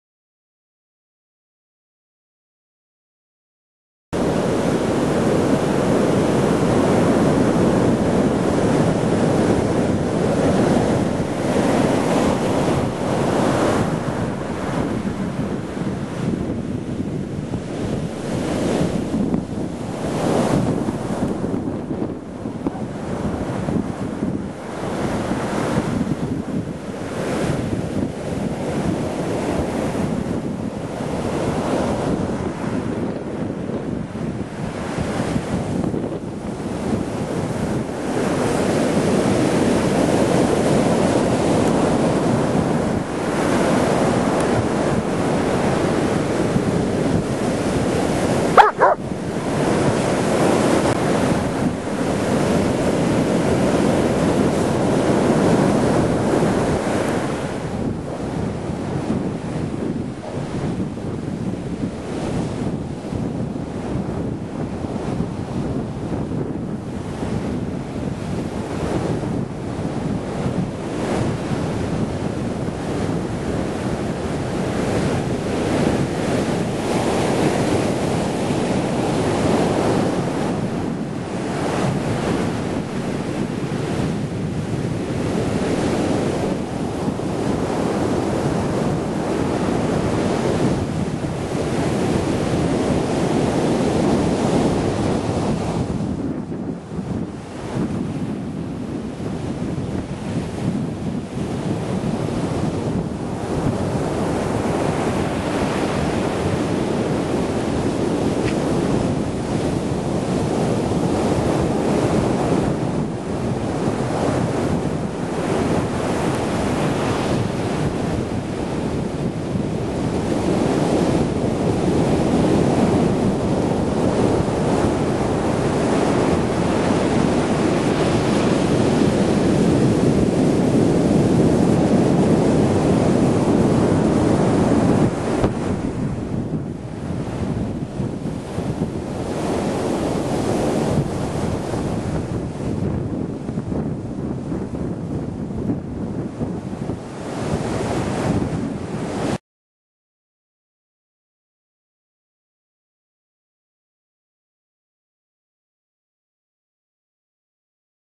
August 25 2011 HURRICANE IRENE Floridas East Coast Dusk Aug 25 2011
This was taken at dusk. Sorry the video is so dark but had to wait untill work was over. It was pretty big wave for the East Coast of Florida. This was taken at Queens Island Park in Fort Pierce FL. Floridas Coast was spared. We got some gusts, sideways rain and lots of feeder bands. I hope the folks in the current path are just as lucky. Please send you comments below and links to your Hurricane Irene Videos.